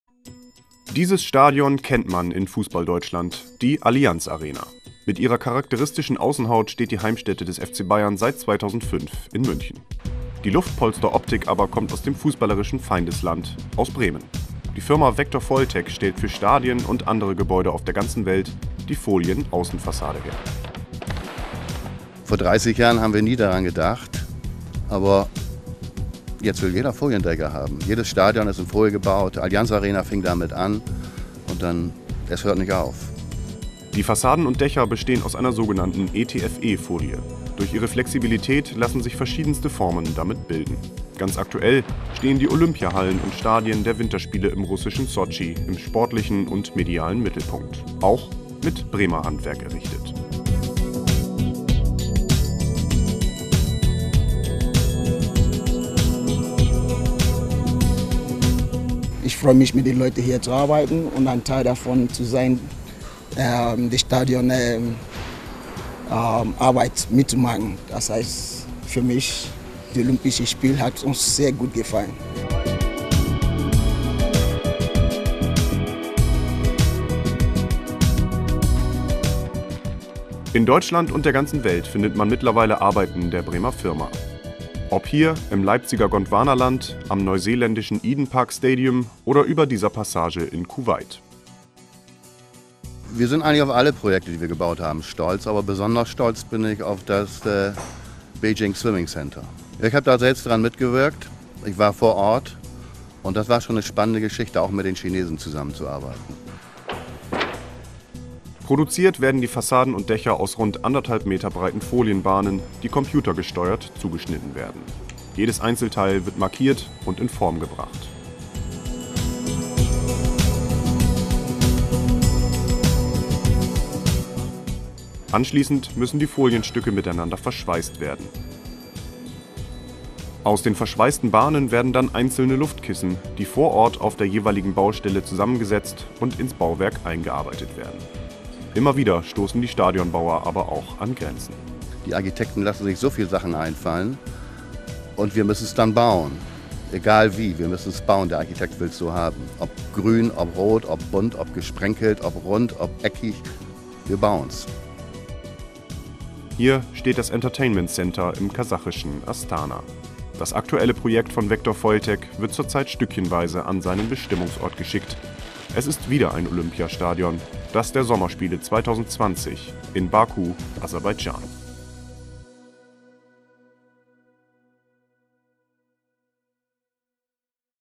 Stadionhüllen made in Bremen
Ob Allianz Arena in München, Olympia-Stadion in Sotschi oder Schwimmhalle in Peking: Sie haben alle die gleich Außenhaut - und die komt aus Bremen von der Firma Vector Foiltec.